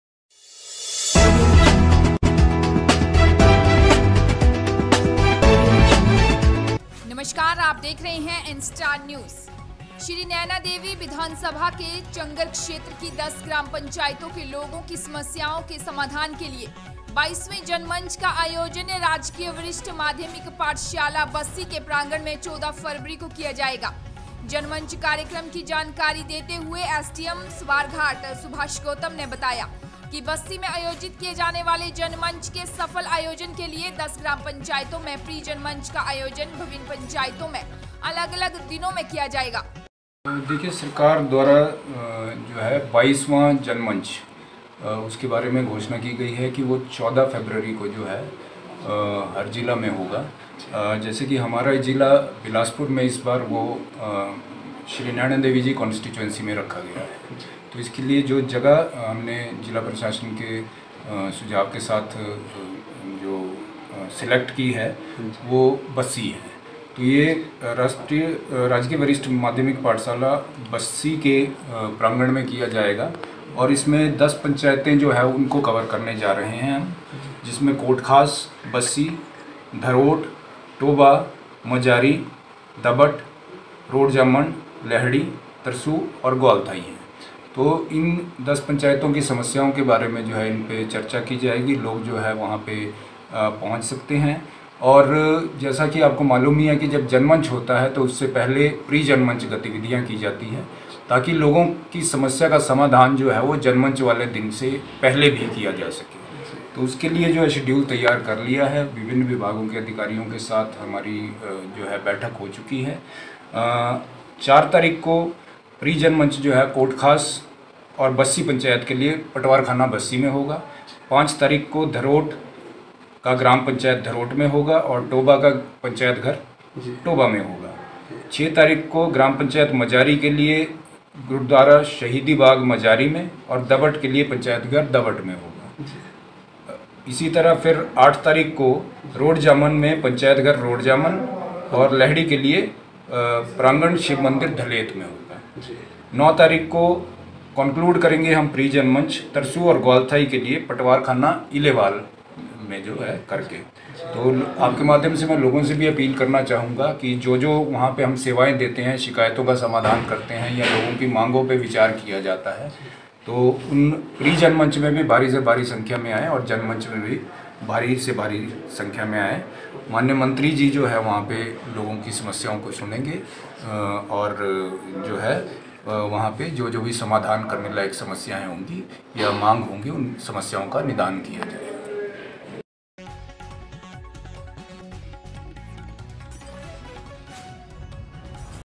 देखें स्वारघाट से मनदीप राणा की ख़ास रिपोर्ट लोगों की समस्याओं के समाधान के लिए 22वें जनमंच क
लोगों की समस्याओं के समाधान के लिए 22वें जनमंच का आयोजन 14 फ़रवरी को